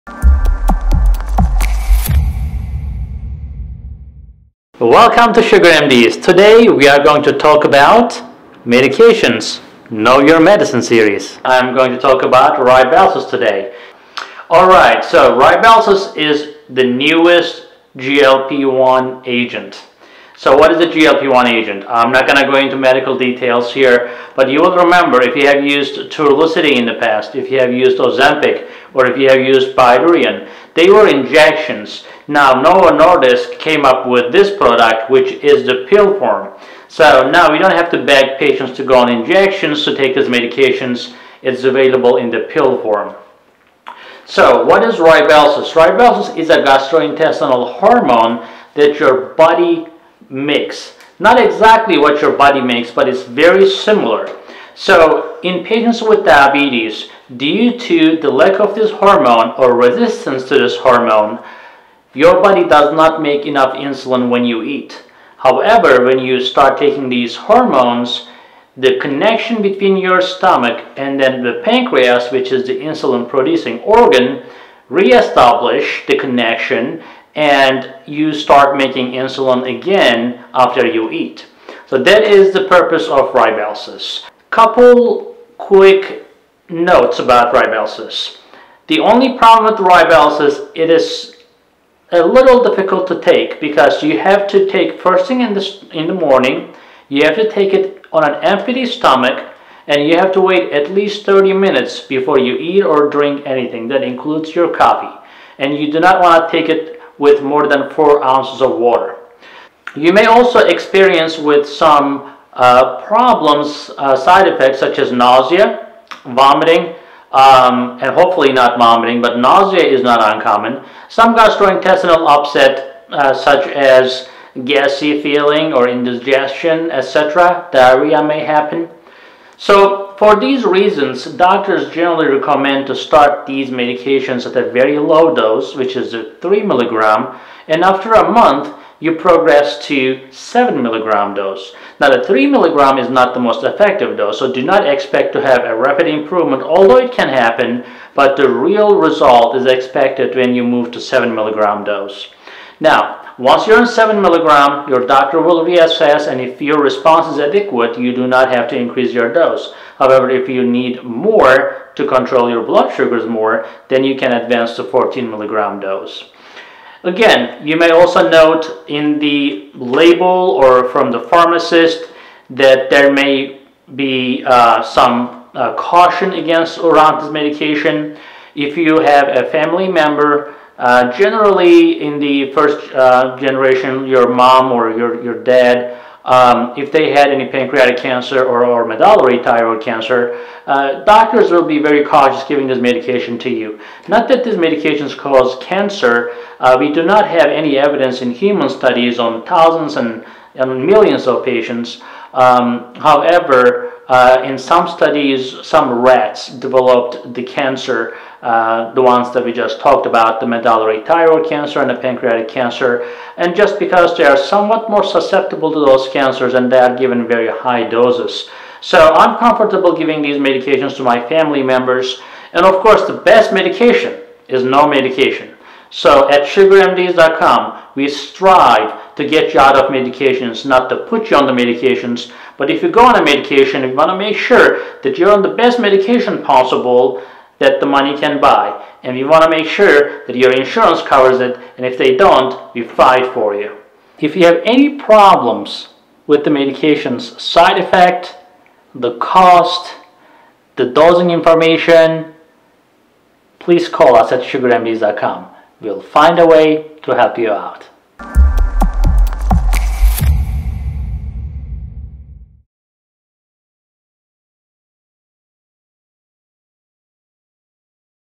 RYBELSUS Review! Does Rybelsus cause Weight Loss in Diabetics? Doctor Explains! SugarMD
Dr. Ergin, founder of SugarMDs tells you everything about Rybelsus every patient with diabetes on Rybelsus must know. Rybelsus was introduced in 2020. It is an oral form of ozempic. It is taken once a day with 4 ounces of water only. You need to wait 30 min without eating or drinking. Always take Rybelsus empty stomach in the morning. It is a strong drug and effective. It can lead to weight loss but also can cause nausea, vomiting, or diarrhea. It does not cause cancer in humans but it did in rats. Rybelsus is one of the most effective medications but it can be expensive without a coupon. If you do not know the price or you need a coupon please let us know. Our patients can get Rybelsus for less than 10 dollars.

𝐅𝐎𝐑 𝐍𝐎𝐍-𝐔𝐒𝐀 𝐂𝐔𝐒𝐓𝐎𝐌𝐄𝐑𝐒 𝐓𝐎 𝐏𝐔𝐑𝐂𝐇𝐀𝐒𝐄 𝐓𝐇𝐄 𝐅𝐎𝐋𝐋𝐎𝐖𝐈𝐍𝐆 𝐎𝐑𝐈𝐆𝐈𝐍𝐀𝐋 𝐀𝐔𝐓𝐇𝐄𝐍𝐓𝐈𝐂 𝐒𝐔𝐆𝐀𝐑𝐌𝐃 𝐏𝐑𝐎𝐃𝐔𝐂𝐓𝐒